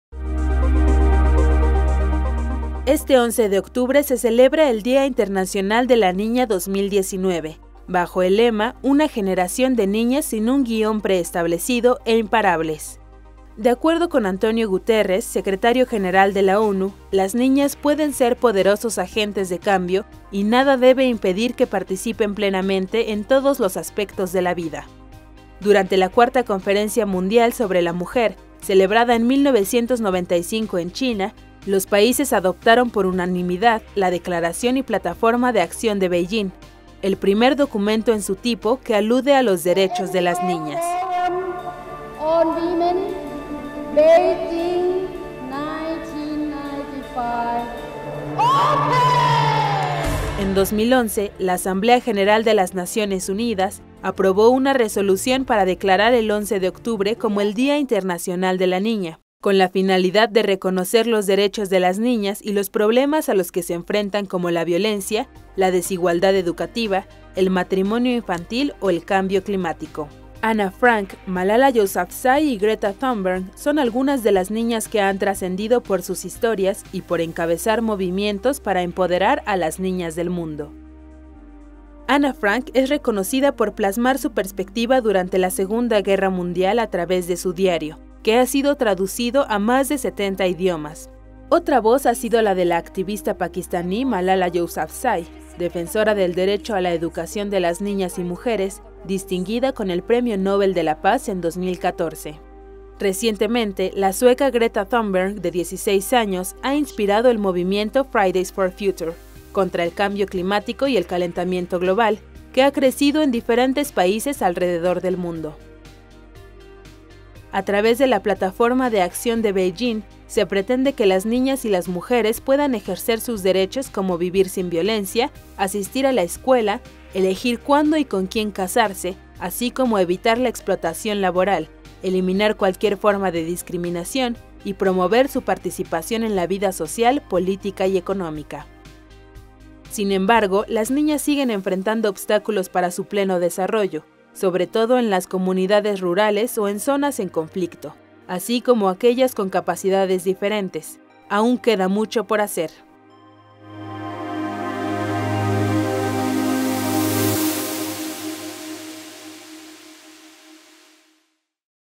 Las niñas, agentes poderosos de cambio
Este 11 de octubre se celebra el Día internacional de la Niña 2019, en el que se reconoce a una generación de niñas que luchan por sus derechos